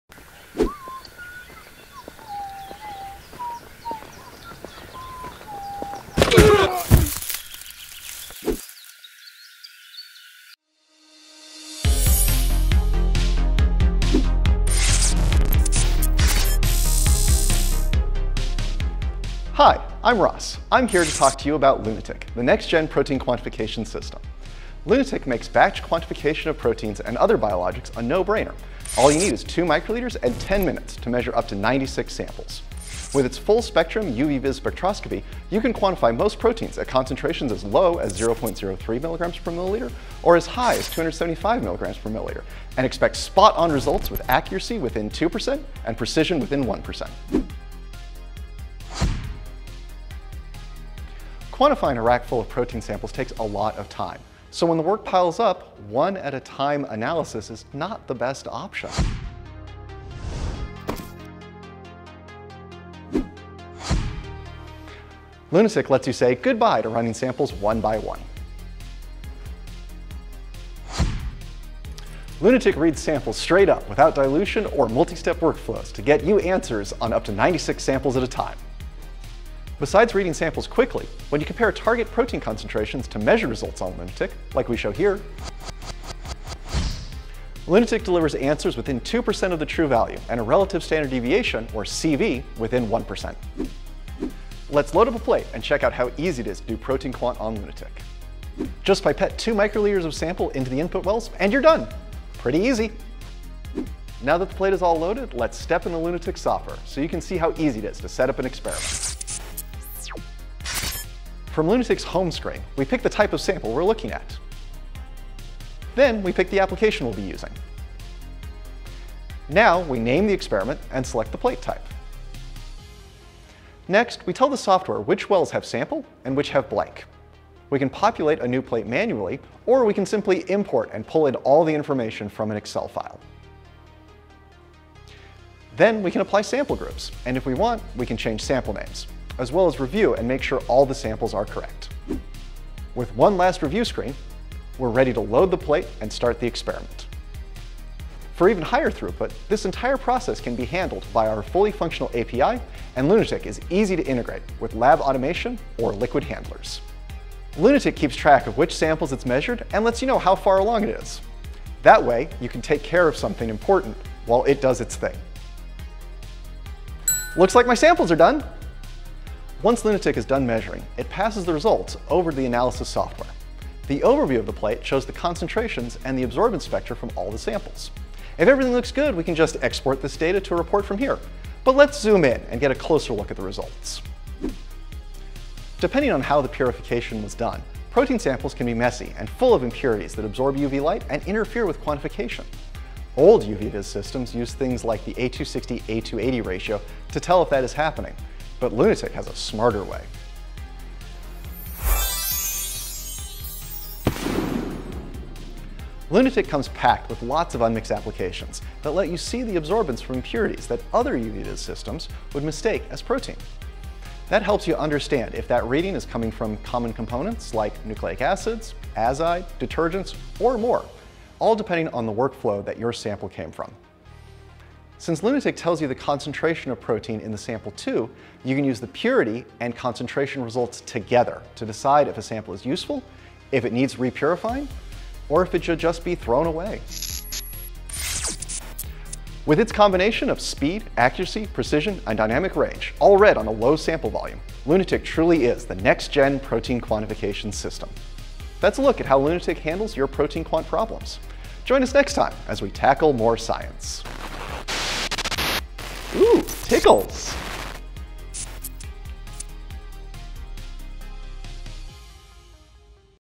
Unchained Tackles Science in 5 - Protein quant with Lunatic
Lunatic is the only system out there that can measure biologics at high-throughput and high concentration. It’s got dynamic range that covers from 0.02 mg/mL to 200 mg/mL (mAb), so run any protein without ever having to dilute again. Stop the madness of running just one protein at a time — and cleaning up afterwards.